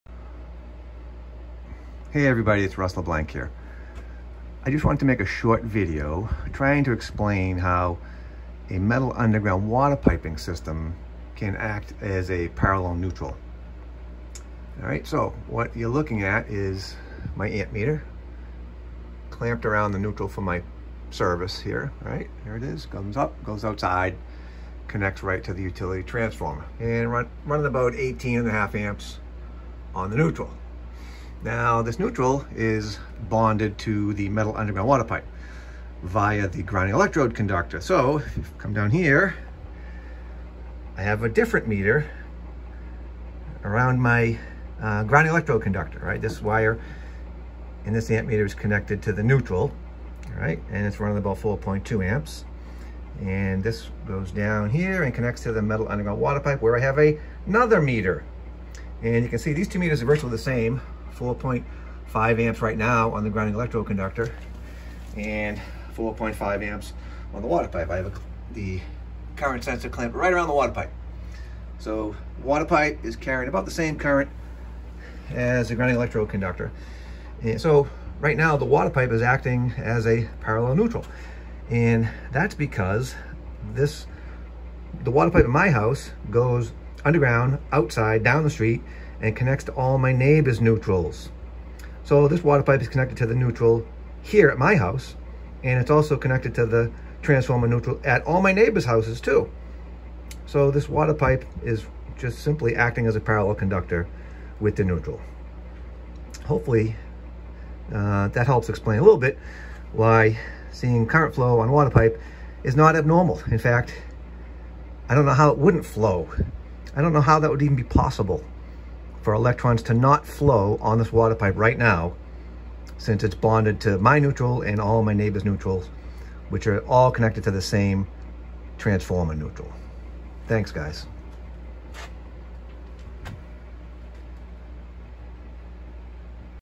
Current on water pipe
A short video explaining why there may be (normal) current flowing on an underground metal water piping system.